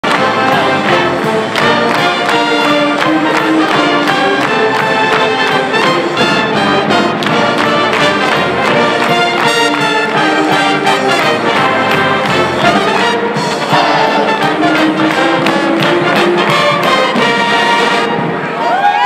UNI men's basketball: Drake vs. UNI Jan. 20, 2013
UNI hosted Drake Jan. 20, 2013.